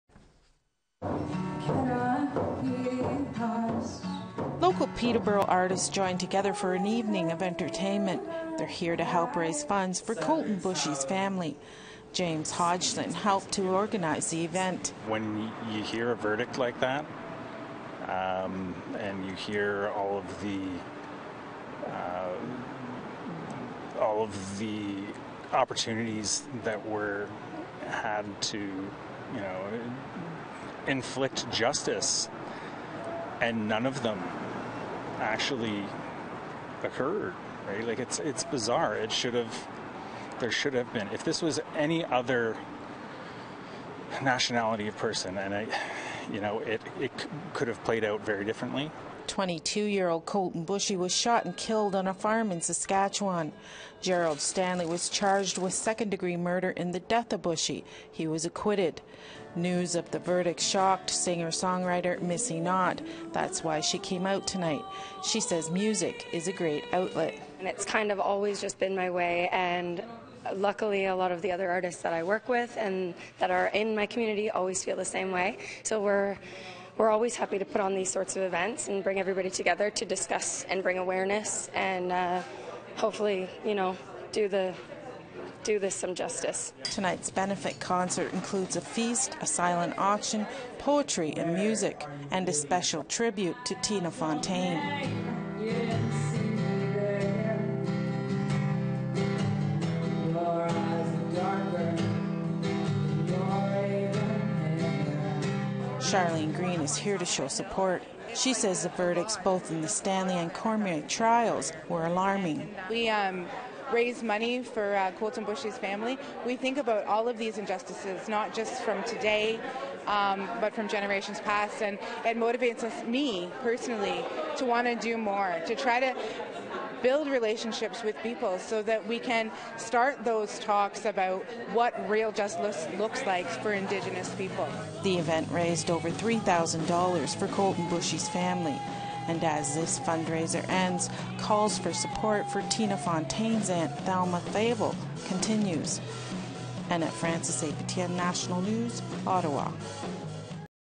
Support for Boushie family continues | APTN News
Its been over two weeks since the Gerald Stanley verdict came down but the support for Colten Bouchie’s family continues.